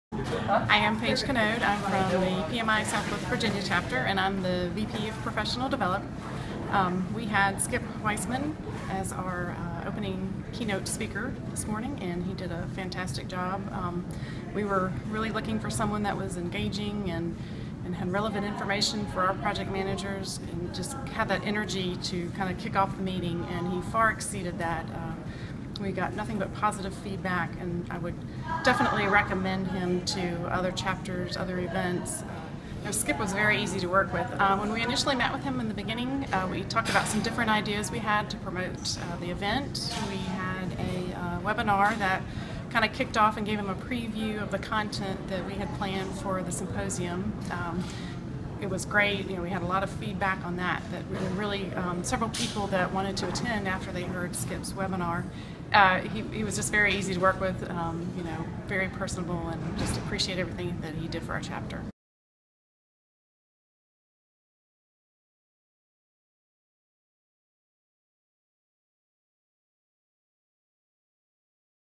Paige Kanode, PMI SW Virginia, PDD Symposium Chair & Speaker Coordinator
Paige speaks to the value Skip Weisman brought to her PMI chapter with his opening keynote address at their chapter's professional development day.